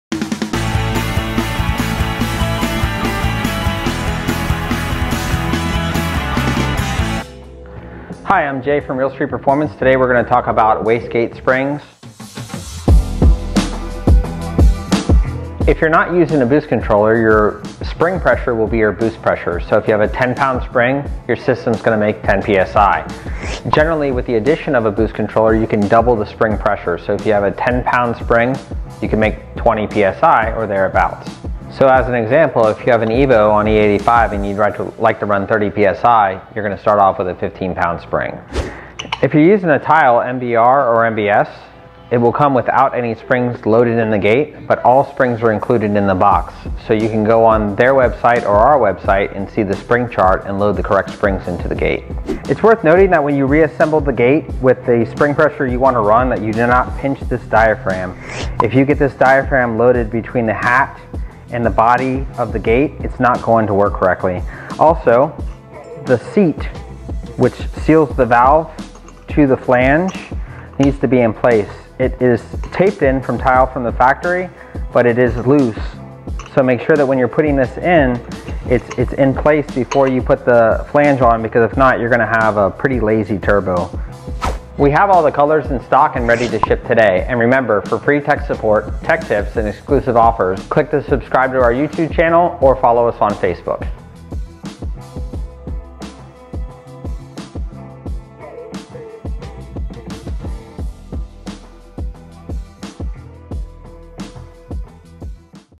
Waste Gate Springs Jay's Tech Tips #11
On this episode of Jay's Tech Tips, Jay helps you select the appropriate wastegate spring for your application. This question pops up weekly, so hopefully this little trick will help.